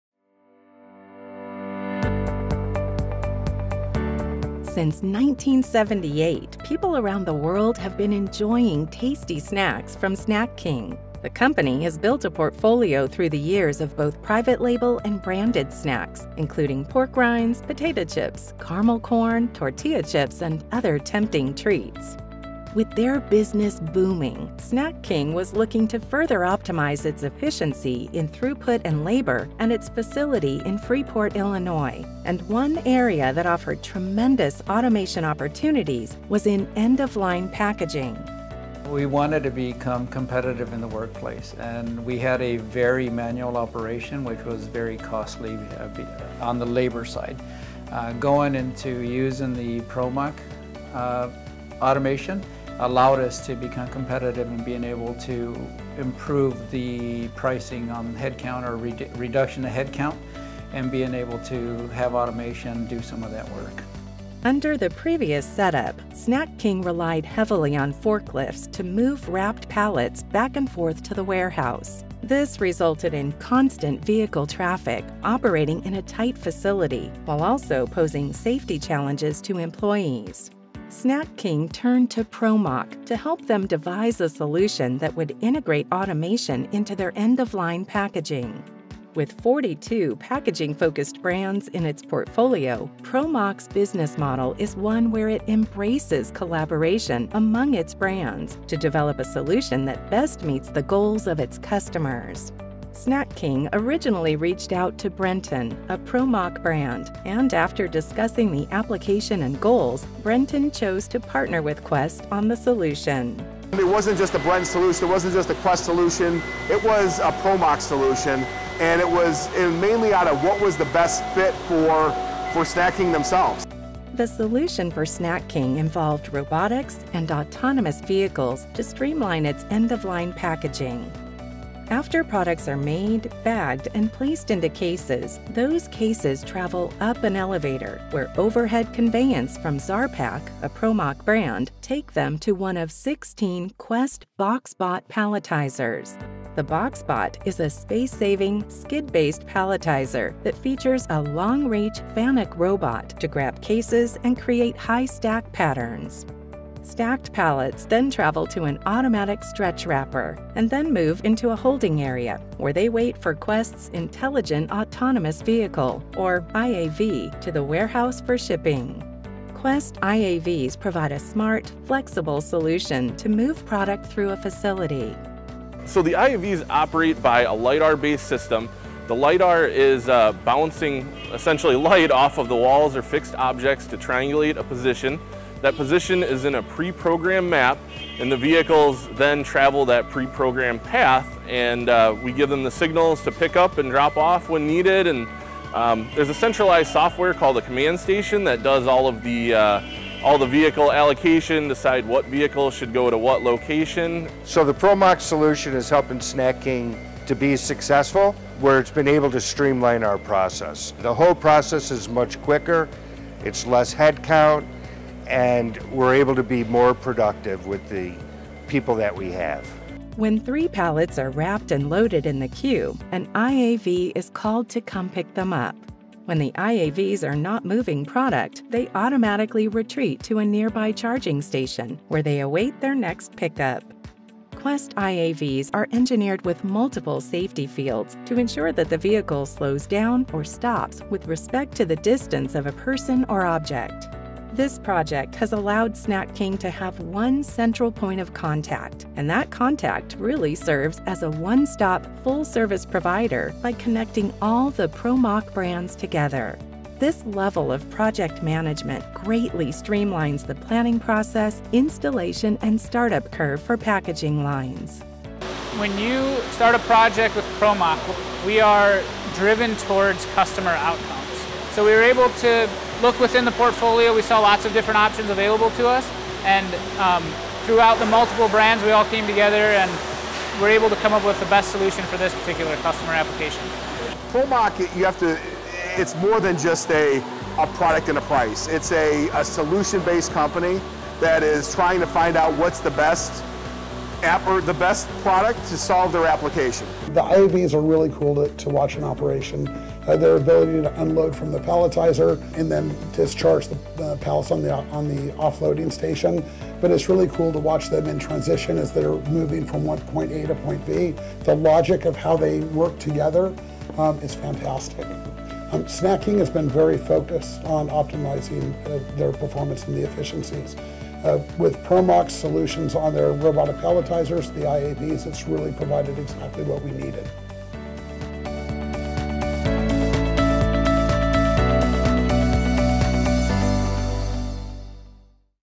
Robotic Palletizing and Autonomous Vehicle Solution for Snack Foods
The solution for Snak King, a leading manufacturer of both private label and branded snacks, involved 16 Box Bot robotic palletizers featuring FANUC robots and three intelligent autonomous vehicles working simultaneously to safely and efficiently move cases of snacks.

After products are made, bagged and placed into cases, those cases travel up an elevator where overhead conveyance from Zarpac, a ProMach brand, take them to one of 16 Quest Box Bots robotic palletizers.

After pallets stacked eight cases high are made, they travel down a centralized trunk line to an automatic stretch wrapper, and then move into a holding pen where they wait for a Quest Intelligent Autonomous Vehicle (IAV) to take them to the warehouse for shipping. IAVs provide a smart, flexible solution to moving product through a facility.

“ProMach has done a really nice job of delivering the overall automation solution that we were looking for. The interface between the robot and palletizers and the IAVs have been seamless,” said Mark Schieldge, COO of Snak King’s Freeport facility.